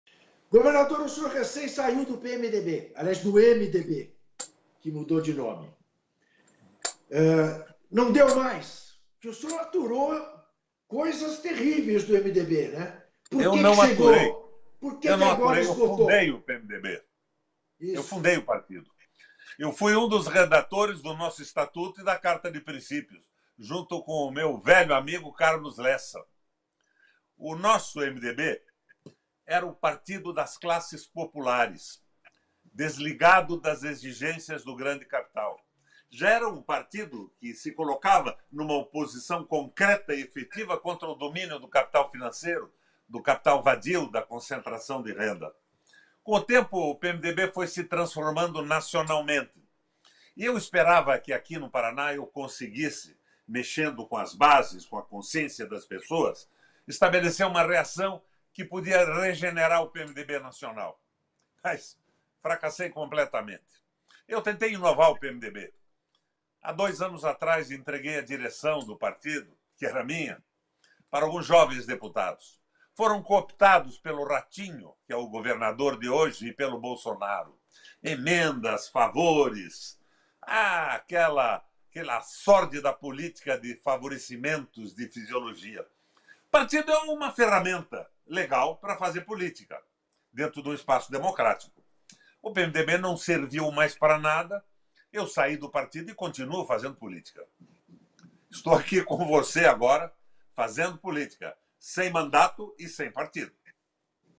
“Saí do MDB, mas continuo fazendo política”
O convidado do Entre Vistas é o ex-governador e ex-senador Roberto Requião.

Em busca de um partido, Requião fala sobre a conjuntura política no Brasil, da atuação do governo Bolsonaro e perspectivas para 2022.